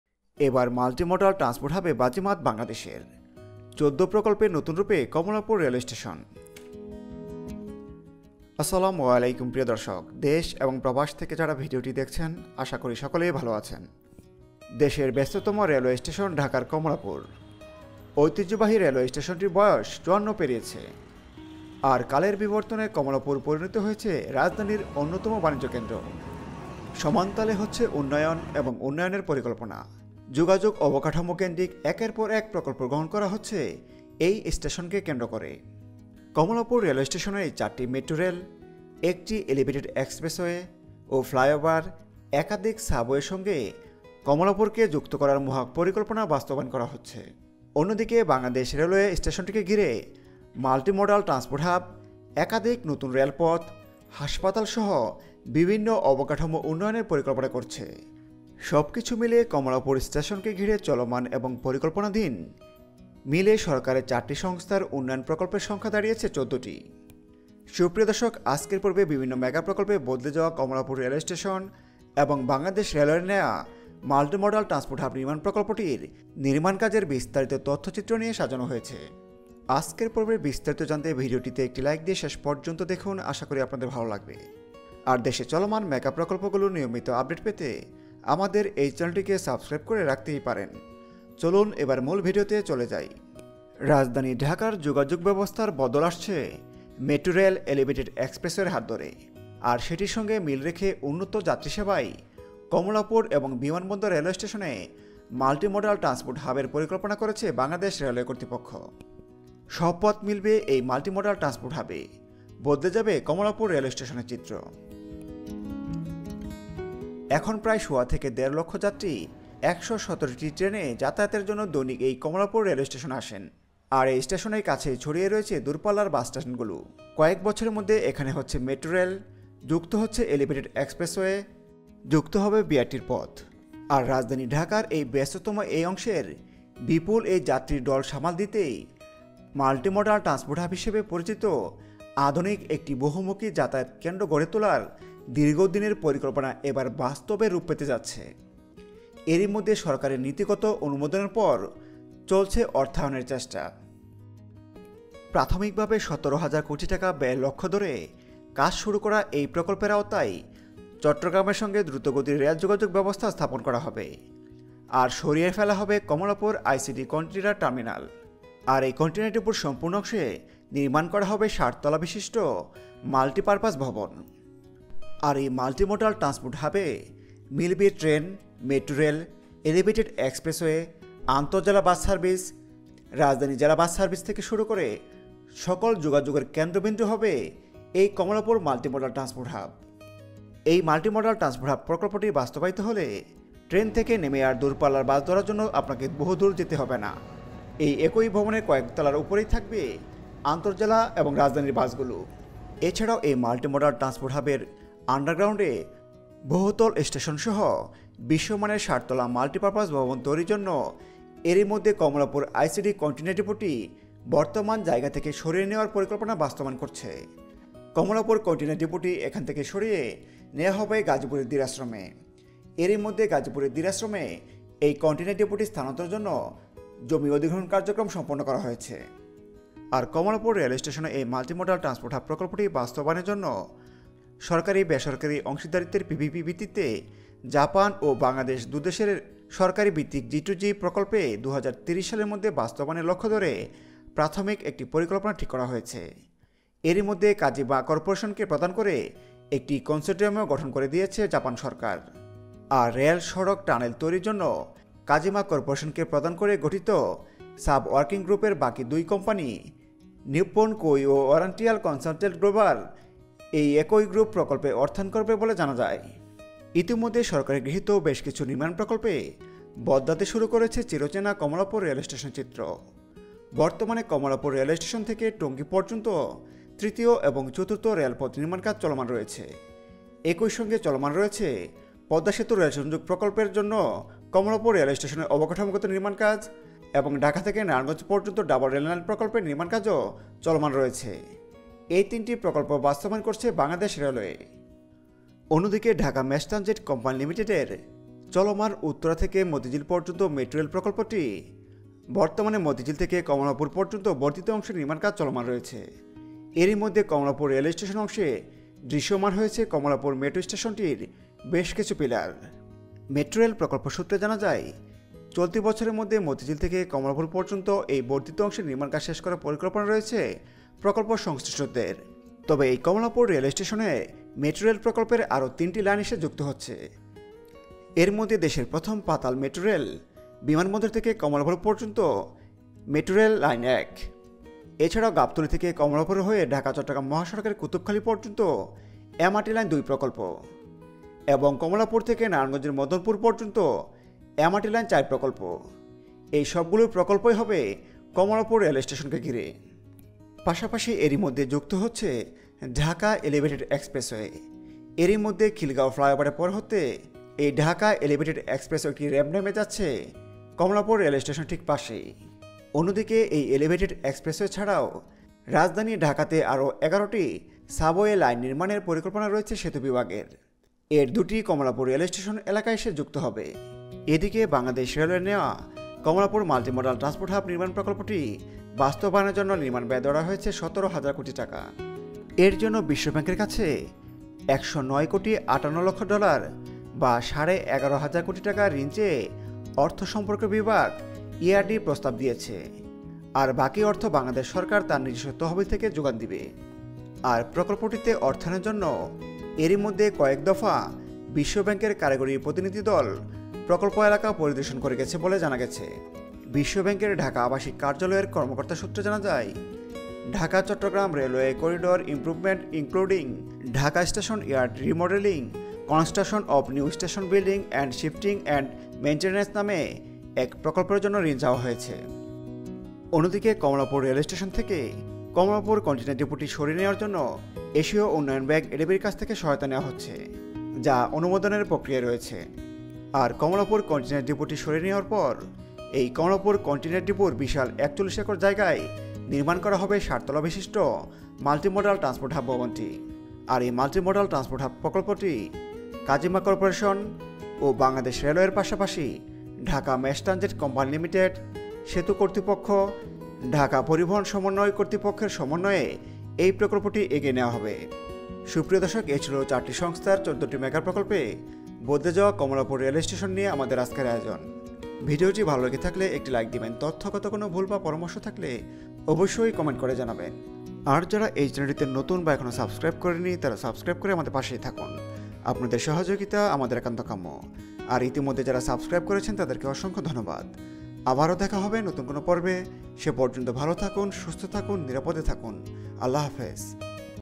🇧🇩 মাল্টিমডাল ট্রান্সপোর্ট হাবে ১৪ প্রকল্পে নতুন রুপে কমলাপুর রেলস্টেশন || kamalapur Railway station
দেশের ব্যস্ততম রেলওয়ে স্টেশন ঢাকার কমলাপুর। ঐতিহ্যবাহী স্টেশনটির বয়স ৫৪ বছর। কালের বিবর্তনে কমলাপুর পরিণত হয়েছে রাজধানীর অন্যতম বাণিজ্য কেন্দ্রে। সমানতালে হচ্ছে উন্নয়ন ও উন্নয়নের পরিকল্পনা। যোগাযোগ অবকাঠামোকেন্দ্রিক একের পর এক প্রকল্প গ্রহণ করা হচ্ছে এ স্টেশন কেন্দ্র করে। র আর এই যানজটের হাত থেকে নগরবাসীকে রেহাই দিতে মেট্রোরেল, উড়ালসড়ক, পাতালরেলপথ, রেললাইন সম্প্রাসারণসহ নানা উদ্যোগ বাস্তবান করে চলেছে। এসব প্রকল্পকে একটি ছাদের নিচে আনতে কমলাপুরে একটি মাল্টিমডাল হাব তৈরির পরিকল্পনা গ্রহণ করা হয়েছে।